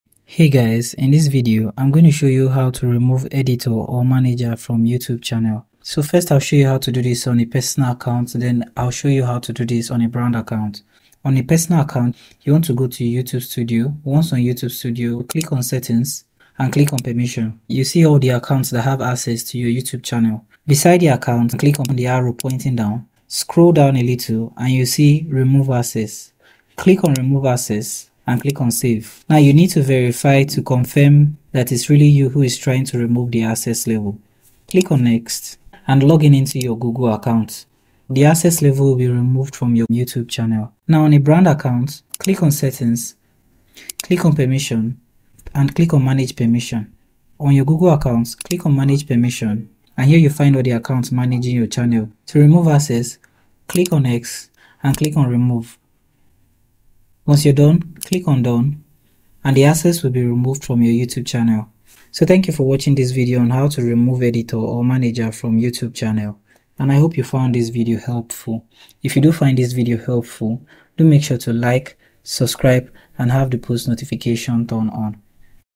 How to Remove Editor or Manager From YouTube Channel - Easy Tutorial
Need to remove an editor or manager from your YouTube channel? In this video, "How to Remove Editor or Manager From YouTube Channel - Easy Tutorial," we’ll guide you through the steps to revoke access from users who no longer need to manage your channel.

Learn how to use YouTube's permissions feature to remove editors or managers, ensuring that only the right people have control over your channel. This tutorial is perfect for creators looking to maintain security and control over their content.

Thanks for joining us! Share these insights with your YouTube community, and let's keep our channels secure together!